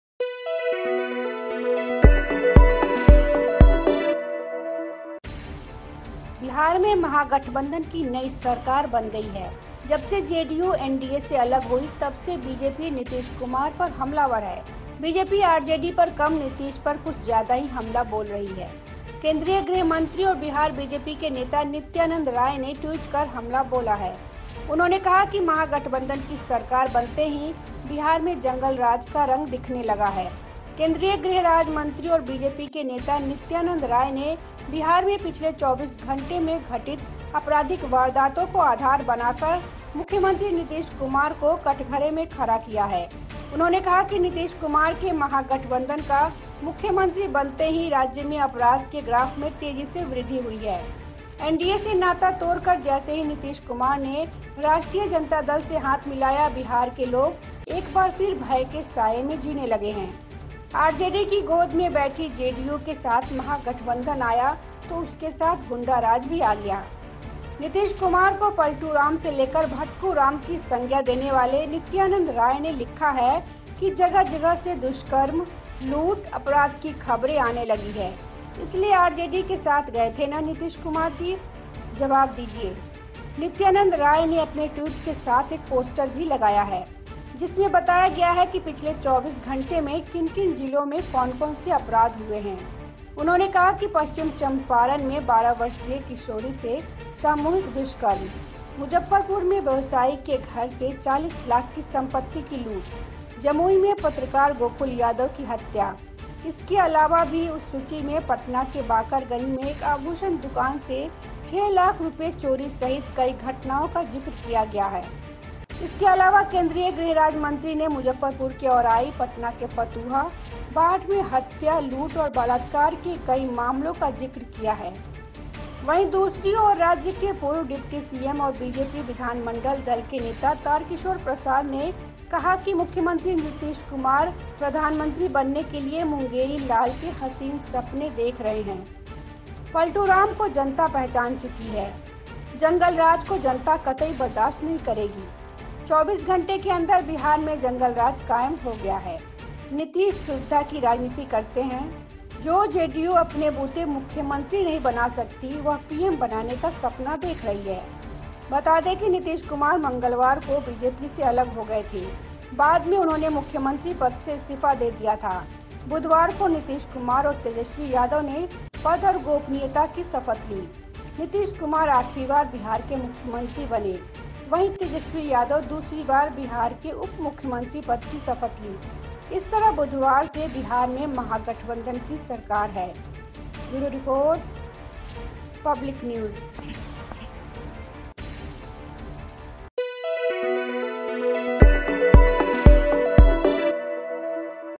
Return of gunda raj in Bihar': BJP lists crimes in 48 hours |Public news.
Within 24 hours of the formation of the Mahagathbandhan government in Bihar, the Bharatiya Janata Party (BJP) released a list of crimes that took place in the last two days claiming that it was an indication of the return of gunda raj in the state.